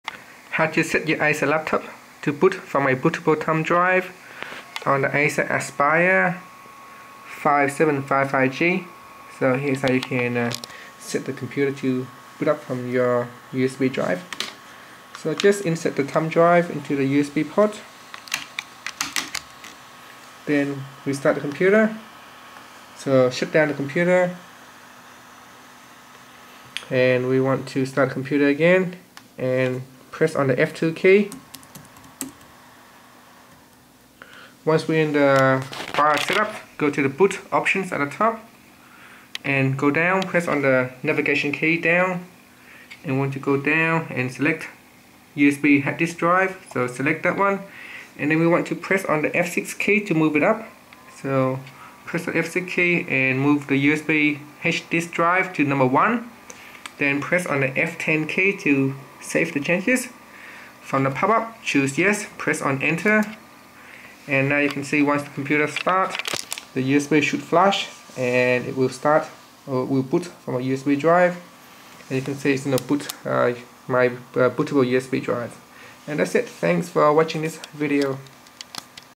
Acer Aspire 5755G: How to Boot From USB Flash Drive
Learn how you can set the Acer Aspire 5755G to boot from USB flash drive on Acer laptop.